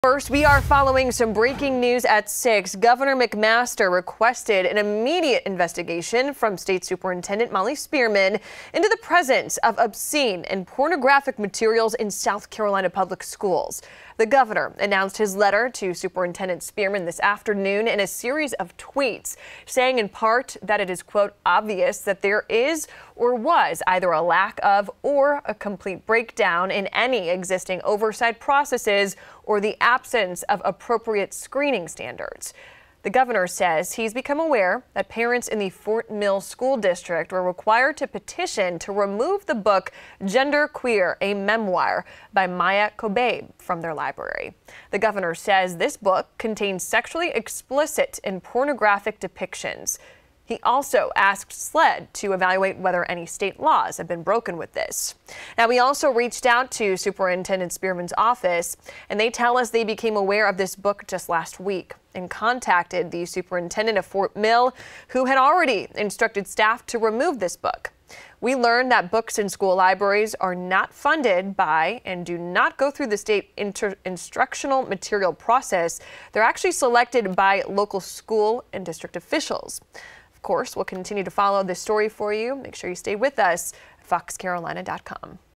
Governor McMaster calls for investigation into books at public schools
The latest on this developing situation